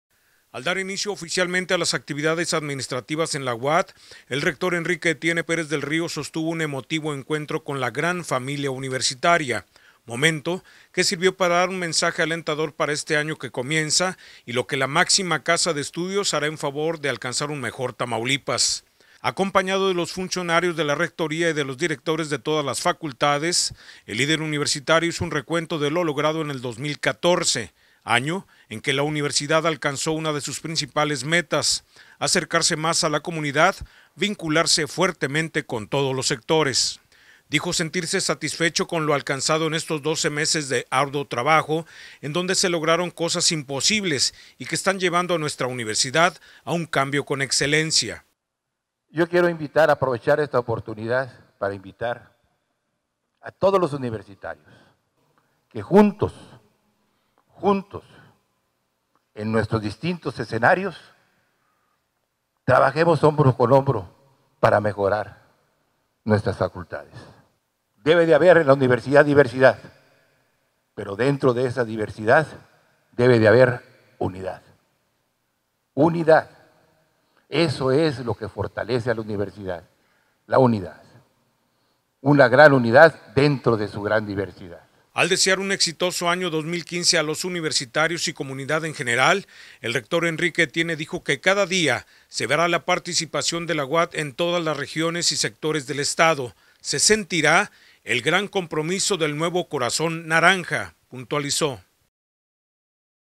Rector invita a universitarios a consolidar valores y trabajar juntos
El Rector Enrique Etienne Pérez del Río de la (UAT, invitó a los universitarios a consolidar valores y trabajar juntos.